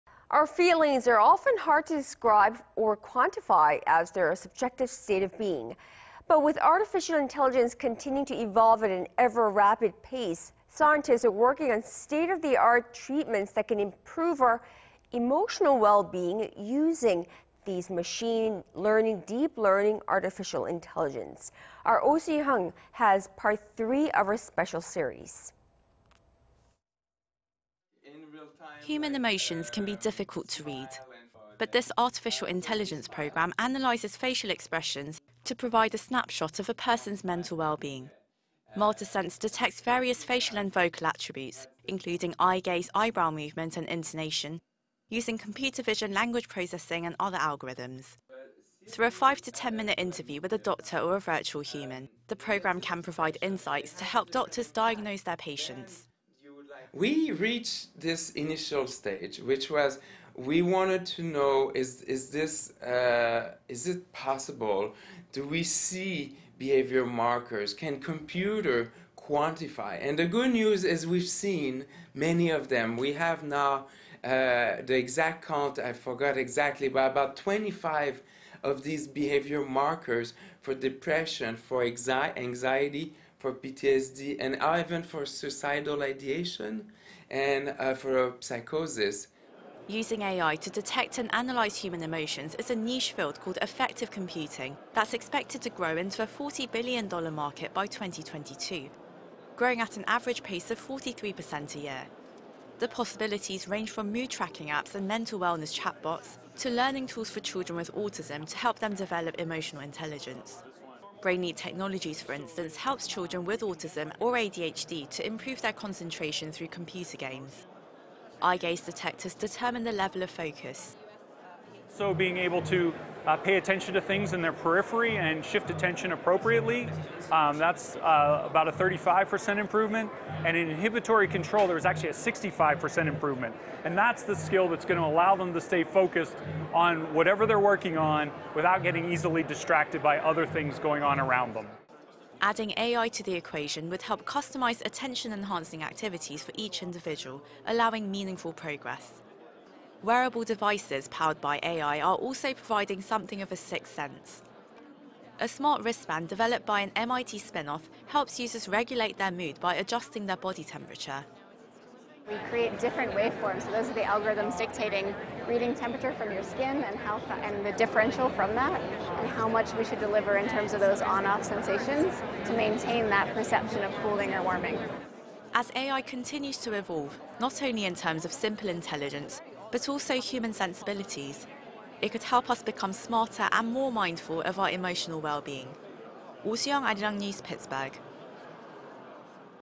AI growing in emotional intelligence, helping diagnose mental disorders...
인간 감정을 읽는 A.I. 정신질환 진단하고 자폐증 어린이들 학습시킨다

AI growing in emotional intelligence, helping diagnose mental disorders, relieving stress with wrist bands

Our feelings are often hard to describe or quantify, as they're a subjective state of being.
But with AI continuing to evolve at an ever rapid pace, scientists are working on state-of-the-art treatments that can improve our emotional well-being.
Oh Soo-young has more.
Human emotions can be difficult to read.
But this Artificial Intelligence program analyzes facial expressions to provide a snapshot of a person's mental wellbeing.
Multisense detects various facial and vocal attributes including eye gaze, eyebrow movement, and intonation,... using computer vision, language processing and other algorithms.
Through a 5 minute interview with a doctor or a virtual human,... the program can provide insights to help doctors diagnose their patients.
"We reach this initial stage, which was, we wanted to know is, is this, uh, is it possible to receive behavioral markers? Can computer quantify? And the good news is we've seen many of them. We have now... about 25 of these behavior markers for depression. For example, anxiety for PTSD and either for suicidal ideation and uh, for psychosis."
Using AI to detect and analyze human emotions is a niche field called affective computing that's expected to grow into a 40 billion dollar market by 2022,... growing at an average pace of 43 percent a year.
The possibilities range from mood tracking apps and mental wellness chatbots,... that help users  become aware of their emotions, to learning tools for children with autism to help them develop emotional intelligence.
Brainleap Technologies for instance,... helps children with autism or ADHD to improve their concentration through computer games.
Eye gaze detectors determine the level of focus.
"Being able to pay attention to things in their periphery and shift attention appropriately that's about a 35 percent Improvement and inhibitory control. There is actually a 65 percent improvement. And that's the skill that's going to allow them to stay focused on whatever they're working on without getting easily distracted by other things going on around them."
Adding AI to the equation would help customize attention enhancing activities for each individual,... allowing meaningful progress.
Wearable devices powered by A.I. are also providing something of a sixth sense.
A smart wristband developed by an MIT spin-off helps users regulate their mood by adjusting their body temperature.
"We create different waveforms. Those are the algorithms dictating reading temperature from skin, and how fast and the differential from that we how much we should deliver in terms of those on up sensations to maintain that perception of cooling and warming."
As AI continues to evolve, not only in terms of simple intelligence, but also human sensibilities,... it could help us become smarter and more mindful of our emotional well-being.
Oh Soo-young, Arirang News, Pittsburgh.